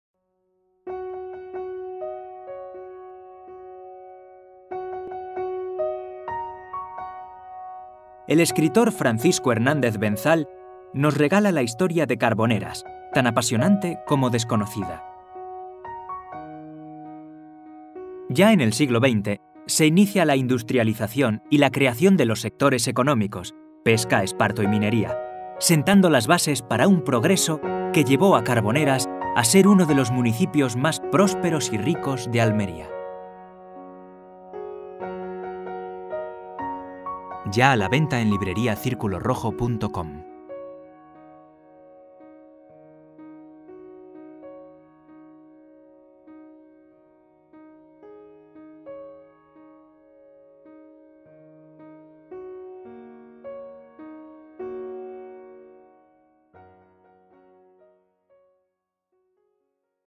Carboneras, un recorrido por su historia (Booktrailer) - Editorial Círculo Rojo
Cuando en el año 2004 comencé a escribir sobre Carboneras, no imaginaba que durante su redacción me encontraría con una historia sobre mi pueblo tan apasionante como desconocida. Todos sabíamos algo sobre su pasado pero no había ningún soporte físico para poder estudiarlo y consultarlo. Se conocían aspectos sobre el Castillo y algunos datos aislados más. Pero Carboneras es mucho más que eso. En la medida que avanzaba sobre el estudio del pueblo, surgían cada vez más interrogantes. Así, comenzamos el recorrido por la historia en pleno siglo XV cuando en su geografía sólo había algún vestigio del paso de los musulmanes. Seguimos cuando los Reyes Católicos entregan este territorio al Condestable de Castilla como compensación por su ayuda en las guerras contra los moriscos. Ya en el siglo XVI, entra en acción el Marqués del Carpio comenzando una labor industrial en las áridas tierras. En el siglo siguiente, el XVII, con la construcción del Castillo comienza la verdadera expansión de la población carbonera. Durante el siglo XVIII se consolida la estrategia defensiva de la costa y se amplía considerablemente la población. En el siglo XIX Carboneras toma forma como municipio independiente de Sorbas y comienza a definir su destino, que en el siglo siguiente, el XX, se inicia con la industrialización y la creación de sectores económicos como la pesca, el esparto, la minería y otros materiales (alumbres). Es esta etapa un periodo que sentó las bases para un progreso en el municipio que conllevó
un aumento progresivo de la población y ser considerado como uno de los municipios más prósperos y ricos de Almería cuando finalizaba el propio siglo. Quiero aprovechar este momento también, en el que todos celebramos el Bicentenario del Ayuntamiento de Carboneras - que reivindico en este libro desde al año 2006 - para celebrar con mis convecinos y amantes de Carboneras tan histórica efemérides.